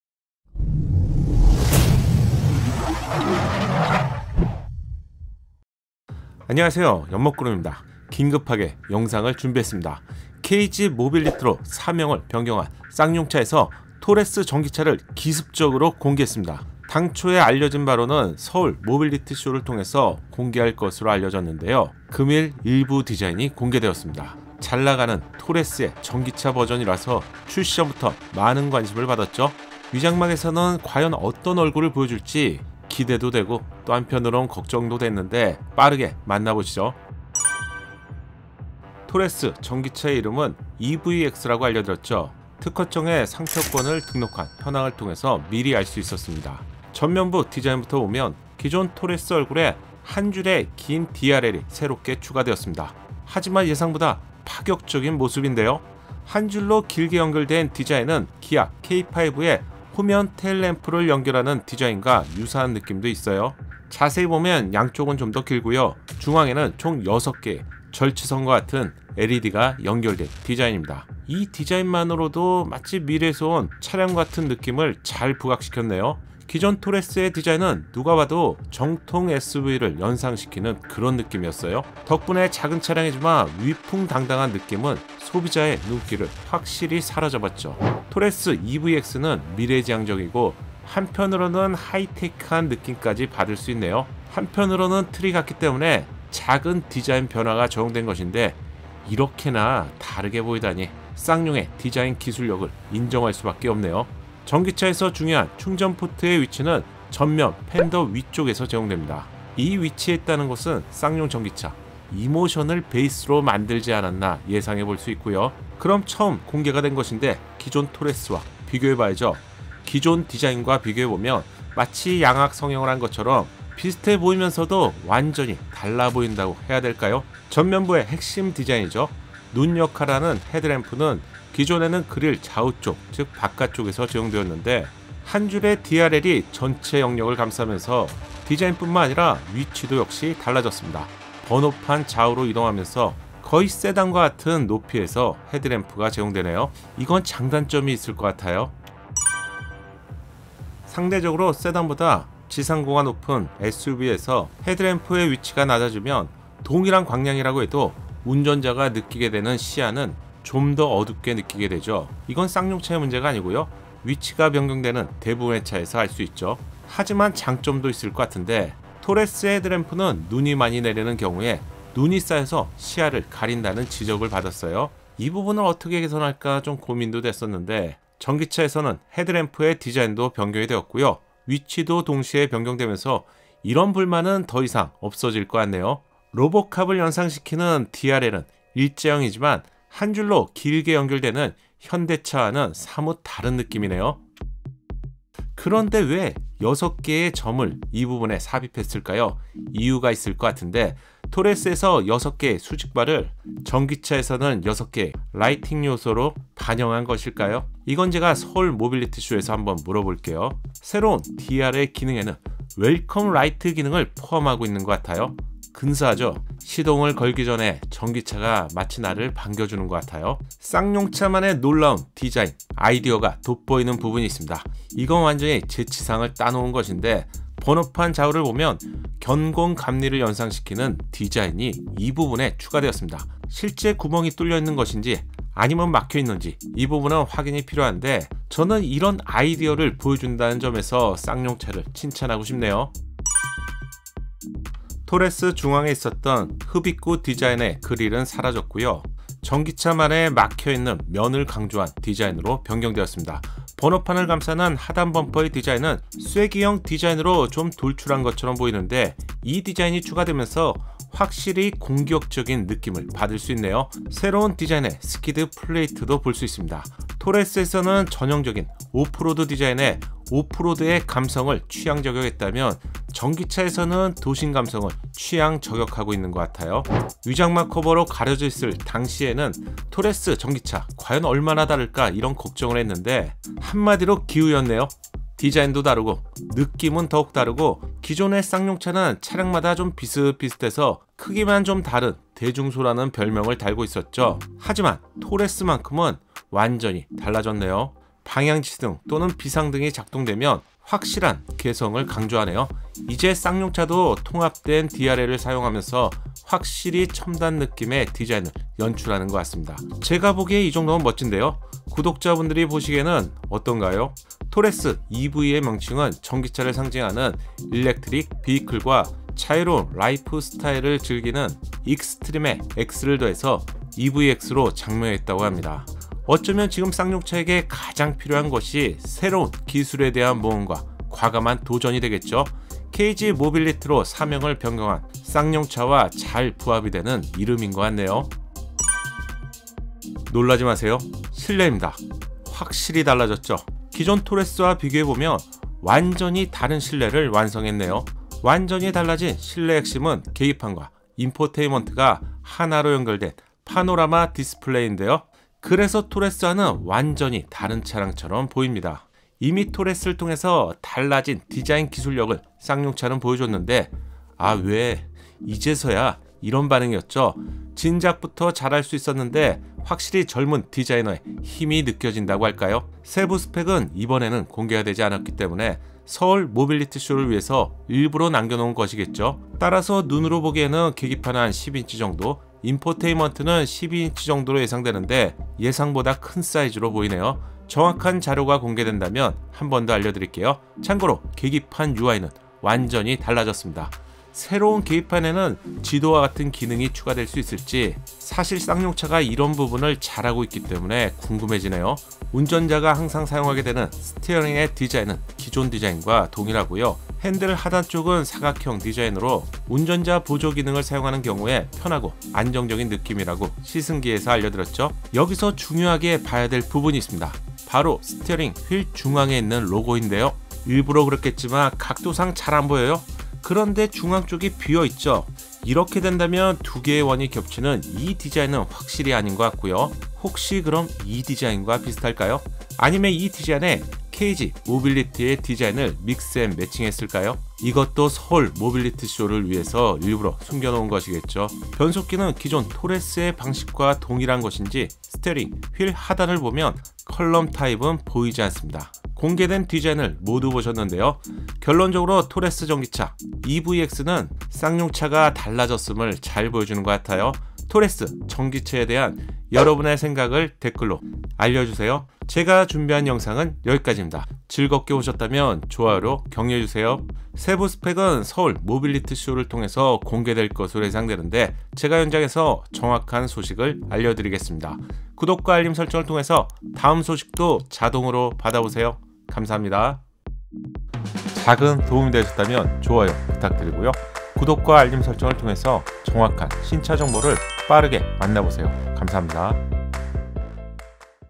토레스 전기차 EVX 공개! 쌍용 작정했다!
쌍용 전기차 EVX가 최초로 공개되었습니다! 쌍용 달라졌네요!

[Video Chapers]